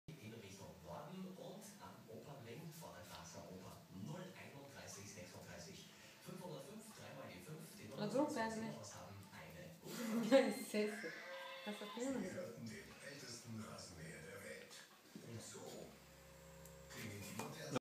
Gucci & Marzipan
Ich bin Gucci ein Cairn Terrier und das ist mein Freund Marzipan. Er liebt es wenn ich seine Ohren kaue!